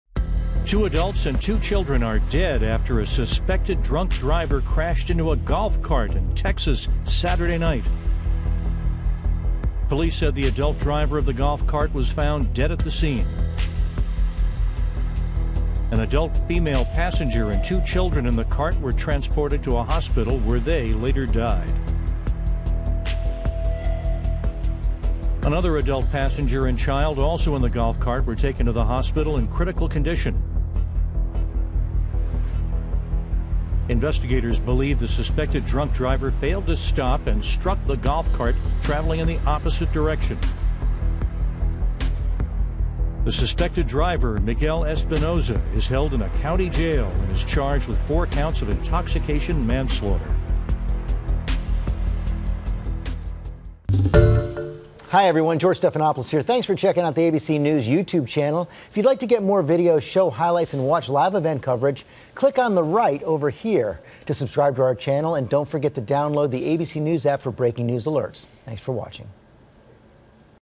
4 dead after suspected drunk driver crashes into golf cart in Texas
The suspected driver, Miguel Espinoza, 45, is currently being held in the Galveston County Jail, according to online records.